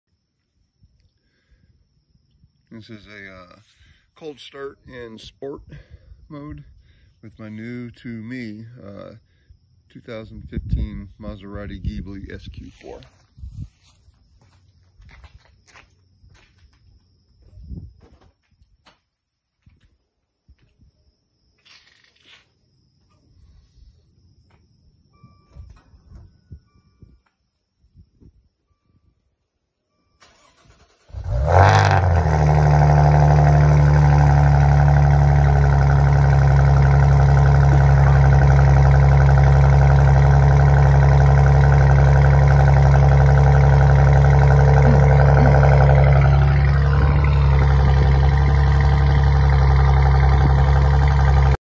2015 Maserati Ghibli SQ4 - Cold start
This is a morning cold start of my new-to-me 2015 Maserati Ghibli SQ4, completely stock.